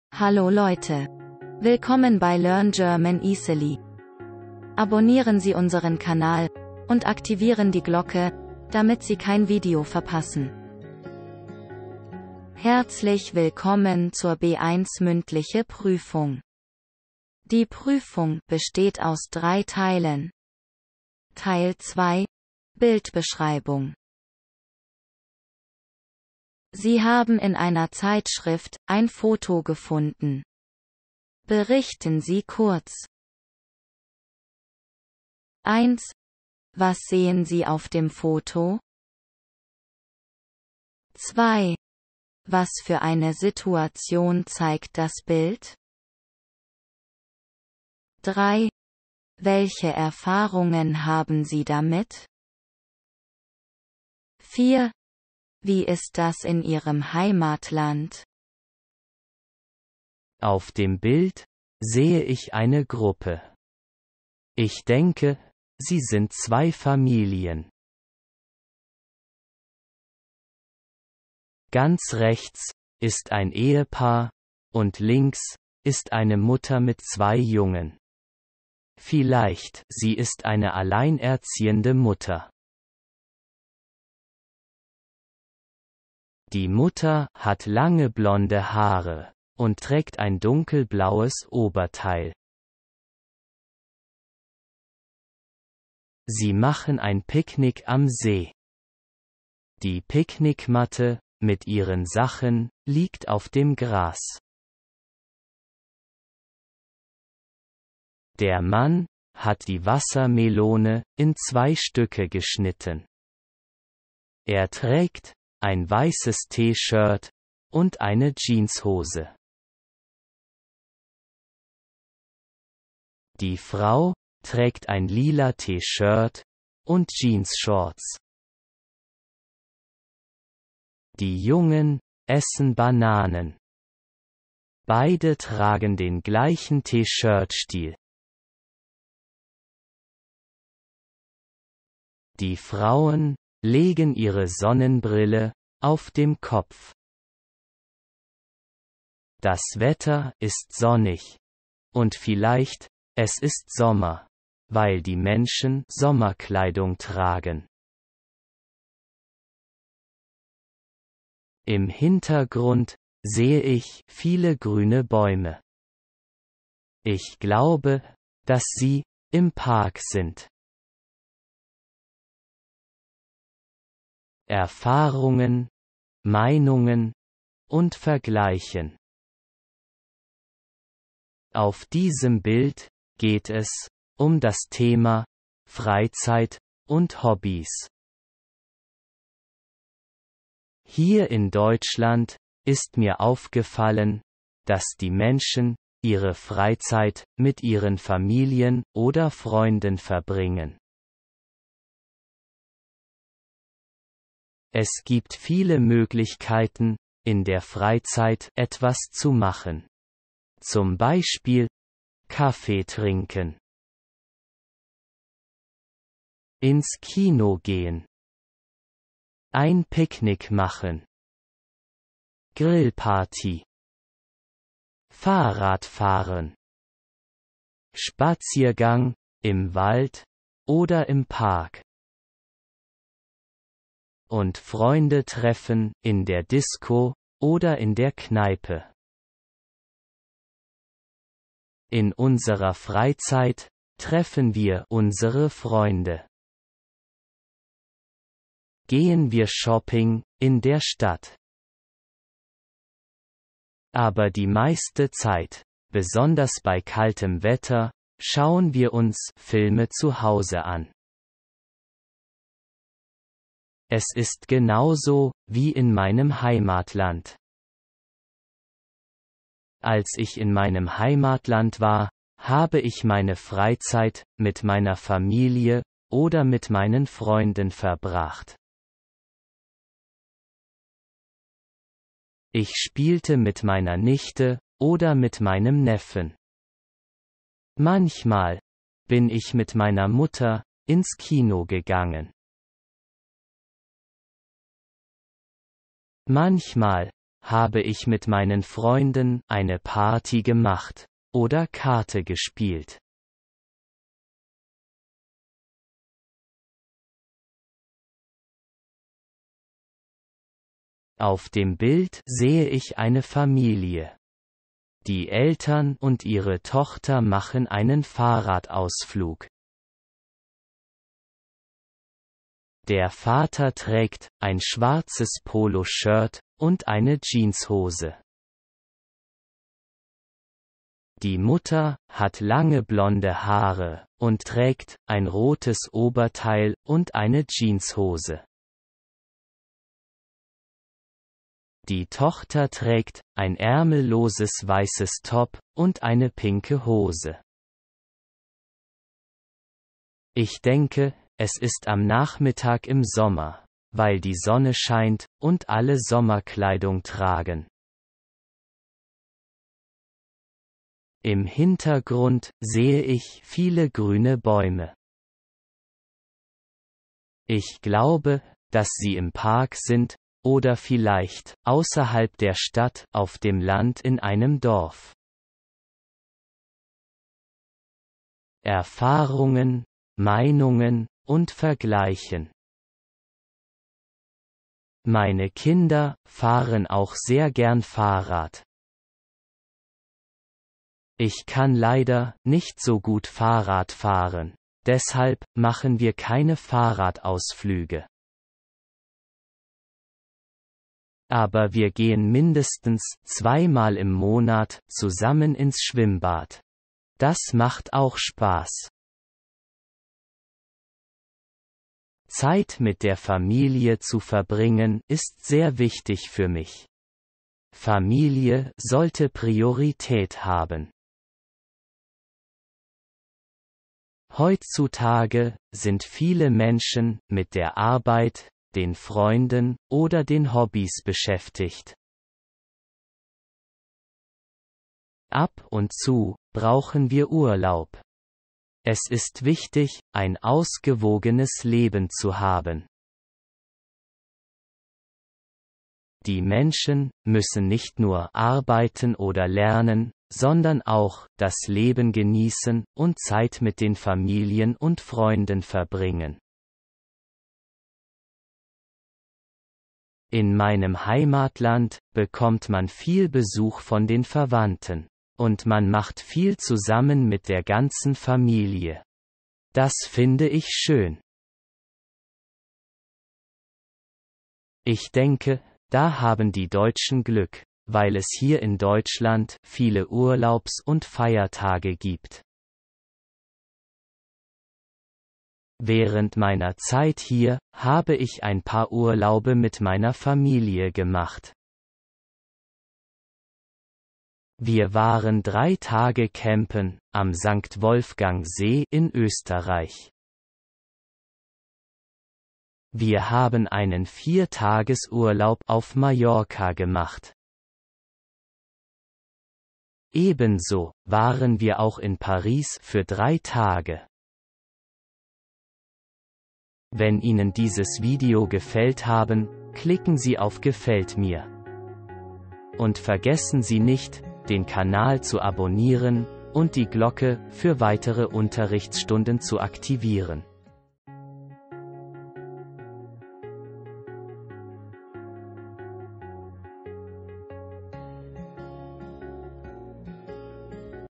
Bildbeschreibung B1 ( DTZ ) Prüfung | Mündliche Prüfung Teil 2
This video shows you how to describe a picture. Anyone taking the B1-Test can get some
interesting clues from this video.

In diesem Video siehst du, wie man ein Bild beschreiben kann.
Wer die B1-Prüfung - DTZ = Deutsch-Test für Zuwanderer - macht kann sich hier für den zweiten Teil der mündlichen Prüfung einige Tipps holen.